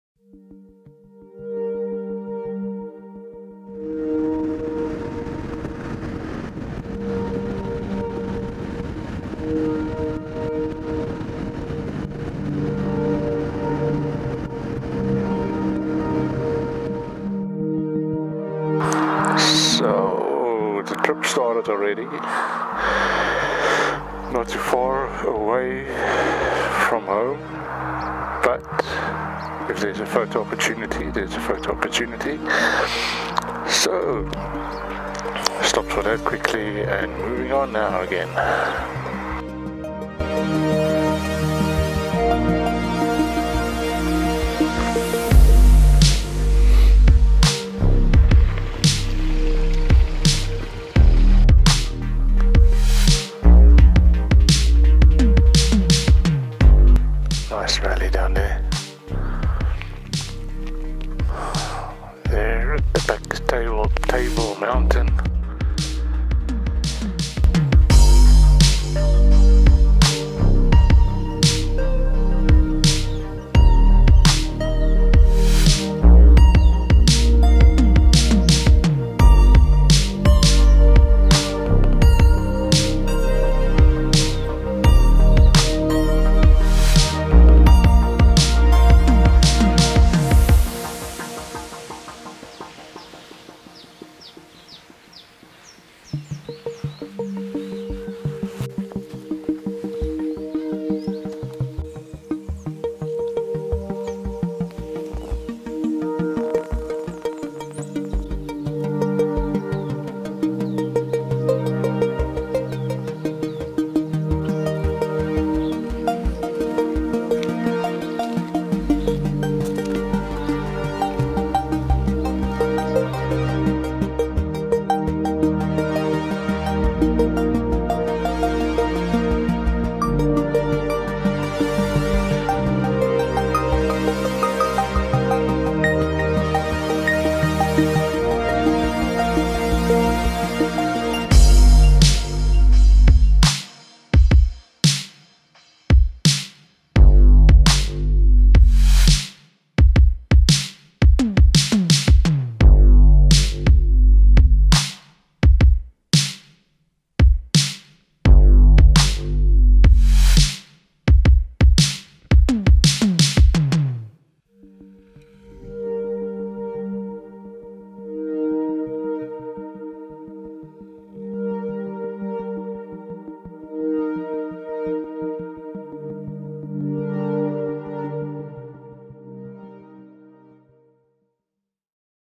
CRF250L + XR125 Winter camp adventure.
My friend and I rode about 140km to go winter camping in the western cape.
The nice thing is that there are only a few other campers, so we could enjoy the quiet nature. A very nice site next to the Berg river.